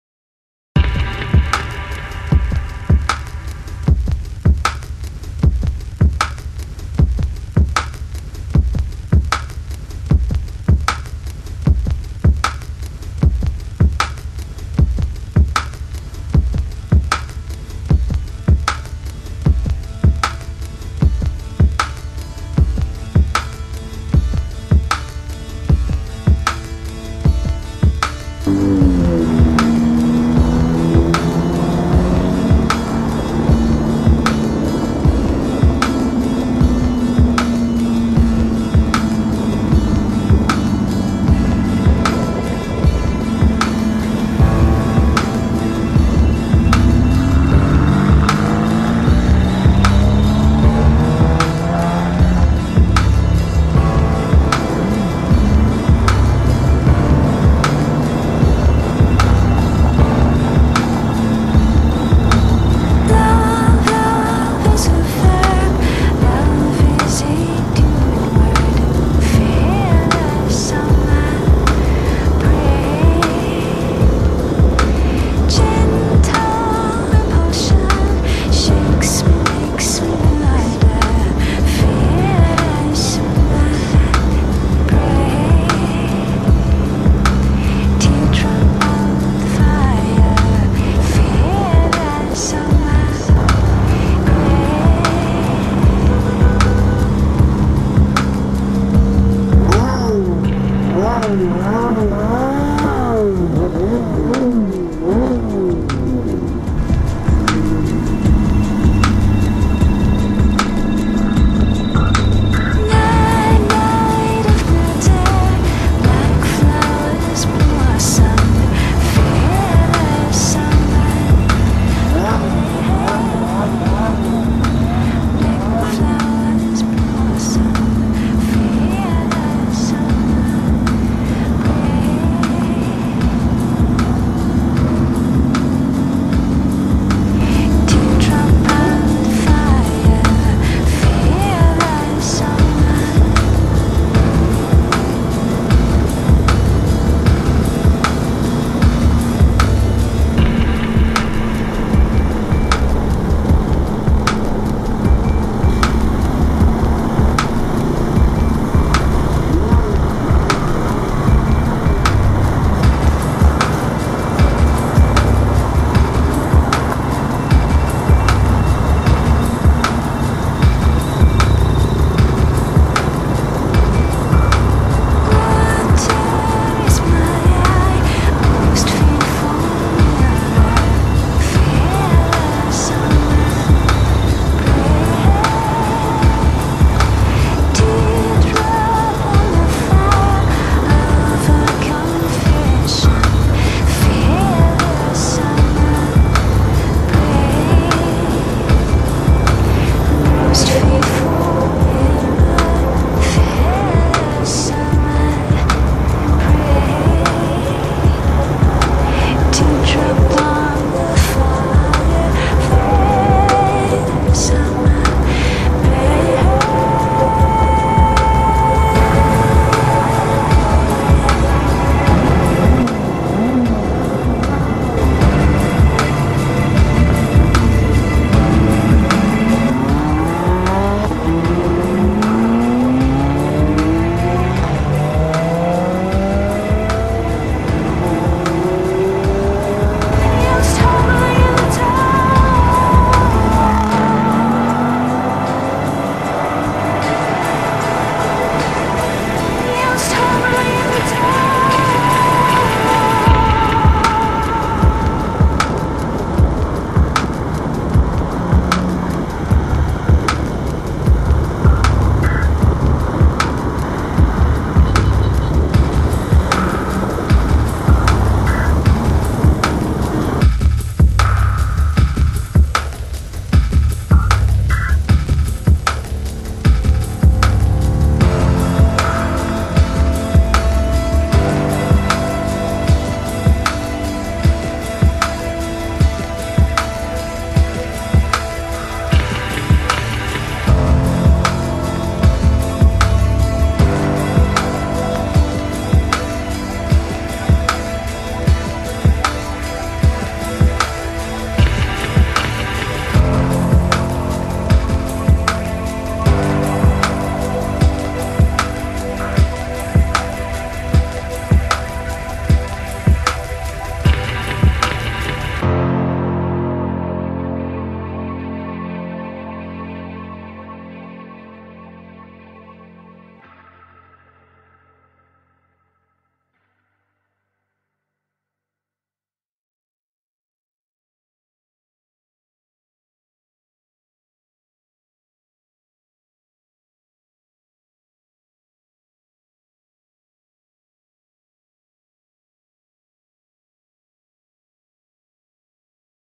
[HD] Supercars in Vietnam 2011 - OF [ Vodka Quiz ]
Hanoi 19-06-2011 ( Siêu xe hội tụ - Hà Nội )
Aston Martin DB9, DB9 Volante, Rapide
Bentley Coninental GT
Ferrari : F430 Spider, 458 Italia , California
Lamborghini Superleggera LP560-4